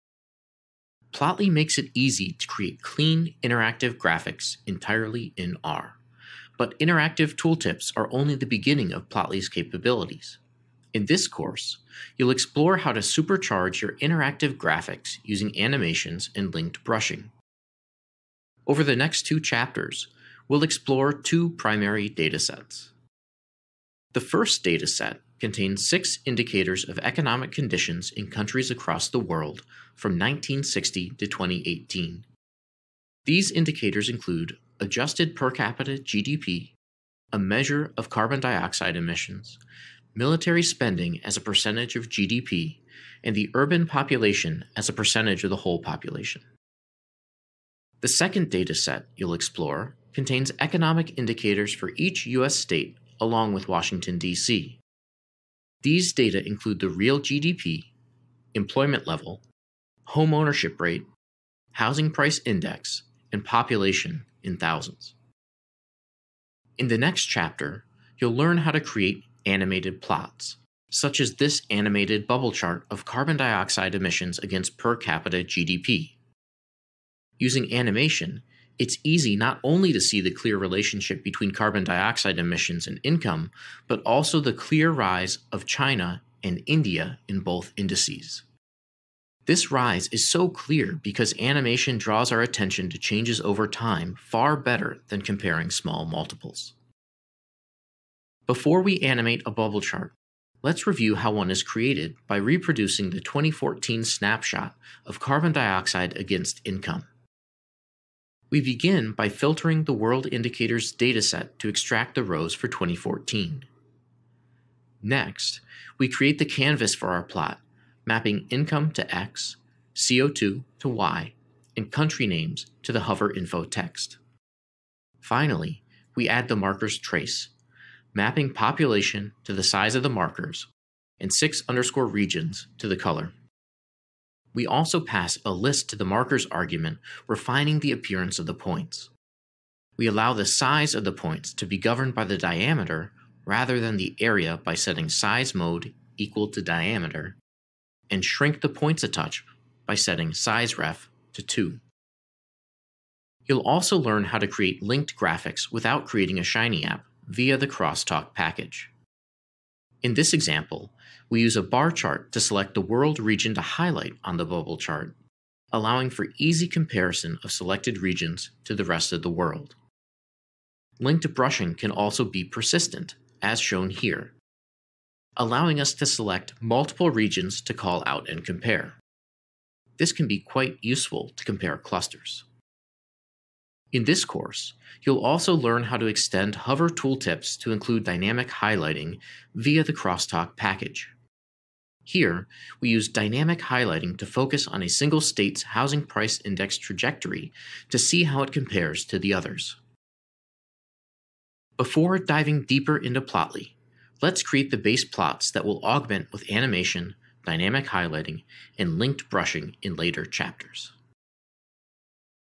R Tutorial: Moving Beyond Simple Interactivity
Want to learn more? at your own pace. More than a video, you'll learn hands-on coding & quickly apply skills to your daily work.

plotly makes it easy to create clean interactive graphics entirely in R, but interactive tooltips are only the beginning of plotly's capabilities. In this course, you will explore how to super-charge your interactive graphics using animations and linked brushing.

Over the next two chapters, we will explore two primary datasets.

The first dataset contains six economic indicators on economic conditions in countries across the world from 1960 to 2017. These indicators include: adjusted per capita GDP, a measure of carbon dioxide emissions, military spending as a percentage of GDP, and the urban population as a percentage of the whole population.

The second dataset you will explore in the next two chapters contains economic indicators for each state in the United States, along with Washington D.C. These data include the real GDP, employment level, homeownership rate, housing price index, and population of each state.

In the next chapter, you will learn how to create animated plots, including the animated bubble chart of carbon dioxide emissions against per capita GDP. Using animation, it's easy not only to see the clear relationship between carbon dioxide emissions and income, but also the clear rise of China and India in both indices.

This rise is so clear because animation draws our attention to changes over time far better than comparing small multiples.

Before we animate a bubble chart, let's review how one is created by reproducing the 2014 snapshot of carbon dioxide against income.
We begin by filtering the world indicators data set to extract the rows for 2014.

Next, we create the canvas for our plot, mapping income to x, co2 to y, and country names to the hoverinfo text.

Finally, add the markers trace, mapping population to the size of the markers and six_regions to the color. We also pass a list to the markers argument refining the appearance of the points. We allow the size of the points to be governed by the diameter rather than the area by setting size mode equal to the diameter and shrink the points a touch by setting sizeref to 2.

You'll also learn how to create linked graphics without creating a shiny app via the crosstalk package. In this example, we use a bar chart to select the world region to highlight the bubble chart, allowing for easy comparison of selected regions to the rest of the world.

Linked brushing can also be persistent, as shown here, allowing us to select multiple regions to call out and compare. This can be quite useful to compare clusters.

In this course, you will also learn how to extend hover tooltips to include dynamic highlighting via the crosstalk package. Here, we use dynamic highlighting to highlight a state's housing price index trajectory to see how it compares to the other states.

Before diving deeper into plotly, let's create the base plots that we'll argument with animation, dynamic highlighting, and linked brushing in the later chapters.